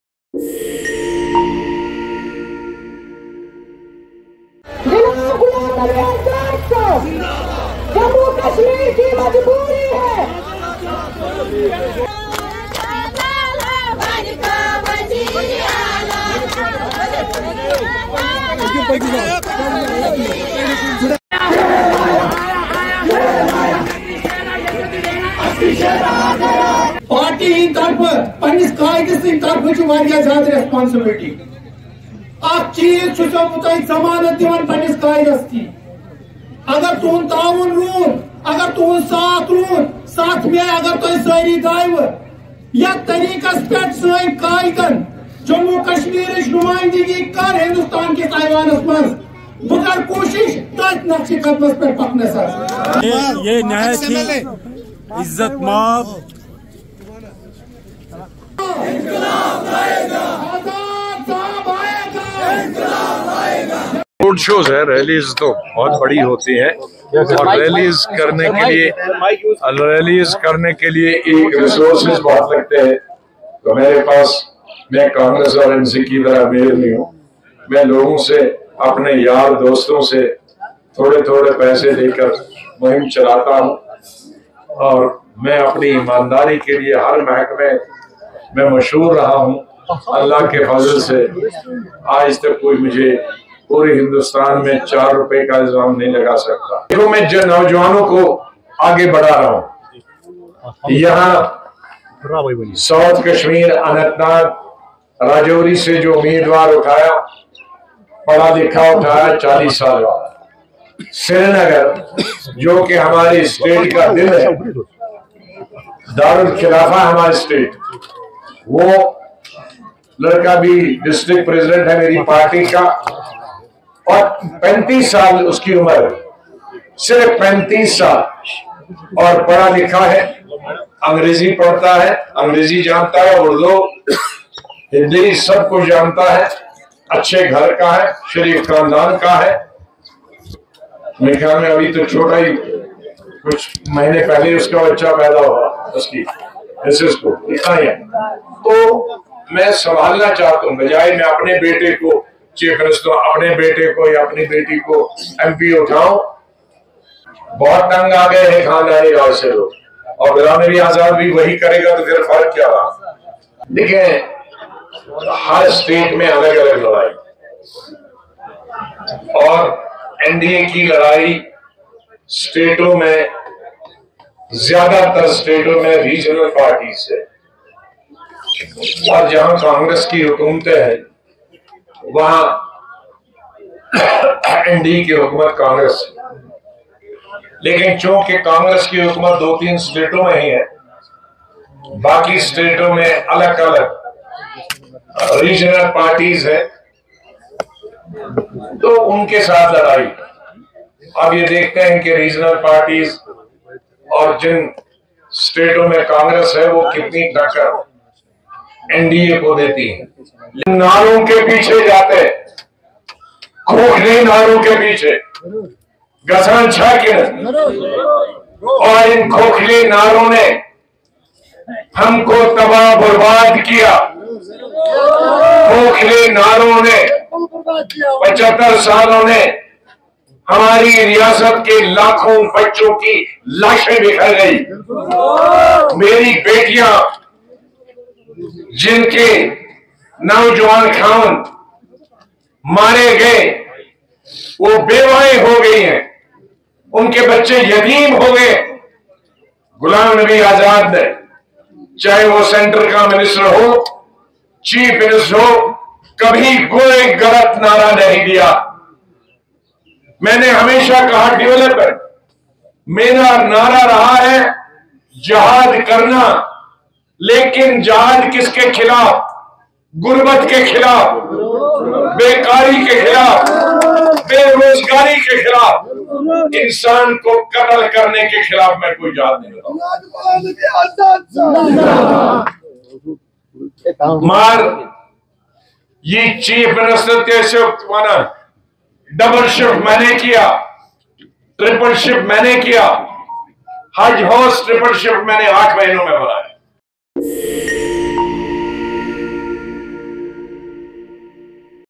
Lok Sabha Elections: We Are bringing Young Faces Forward: Azad || AMC
watch: DPAP Chief Ghulam Nabi Azad while reacting to the candidature of Amir Bhat from Srinagar, Azad said that we have bring young faces forward.
Azad said that The results in the country can be unexpected as NDA has a direct fight with regional parties rather than congress.
The Congress is present in few states and regional parties have a base in most of the states.
Azad was addressing a party road show I  Bulbul Nowgam area of Anantnag.later Ex MLA Pahalgam Abdul Kabir Pathan Joined DPAP in presence of G.N.Azad